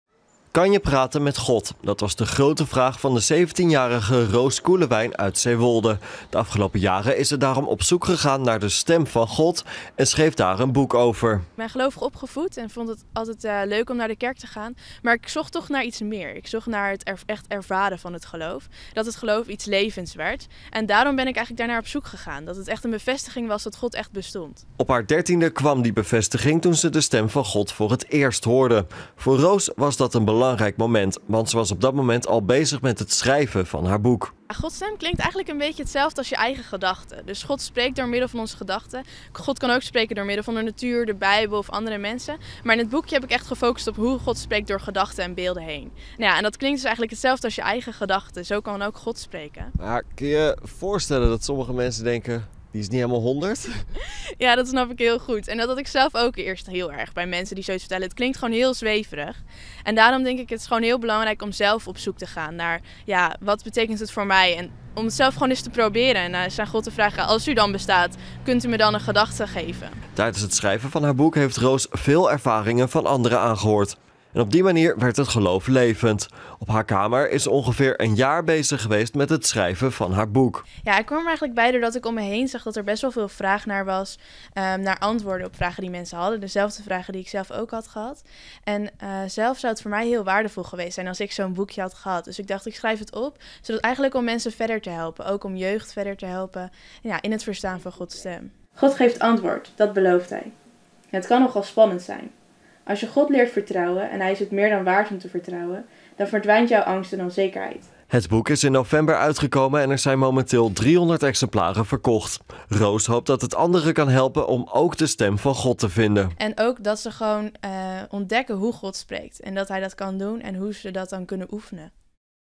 17-jarig meisje schrijft boek over Gods Stem
Kan je praten met God? Dat was de grote vraag van de 17-jarige Roos Koelewijn uit Zeewolde. De afgelopen jaren is ze daarom op zoek gegaan naar de stem van God. Over haar zoektocht en de mensen die ze vanuit de kerk sprak over hun ervaringen schreef ze een boek: 'God spreekt! Hoor jij Hem ook?'.